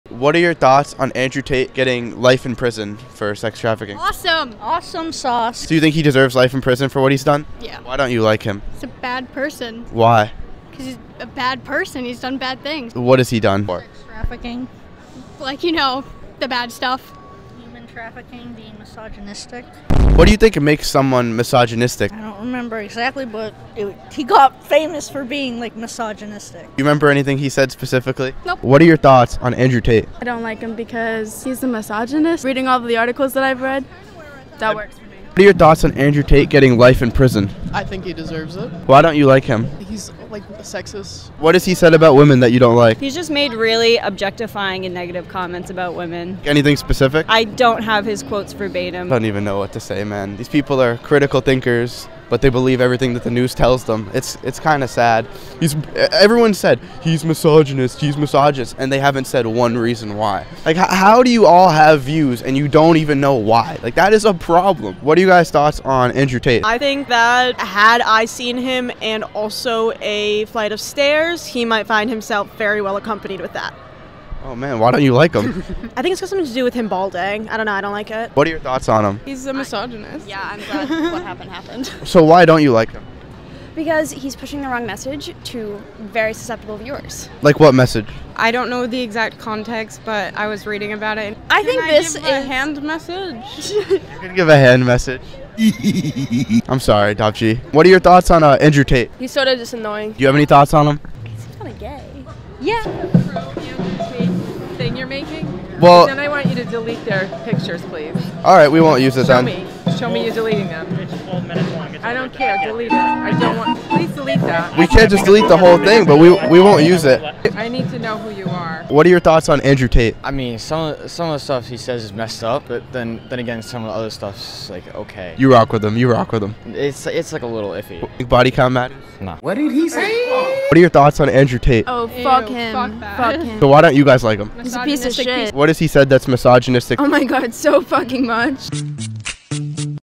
Stupid People Compilation (Andrew Tate) version
These are the types of people hating on Andrew Tate. They don't even have a reason of why they hate him. They are actual NPCS that are fully brainwashed by the media...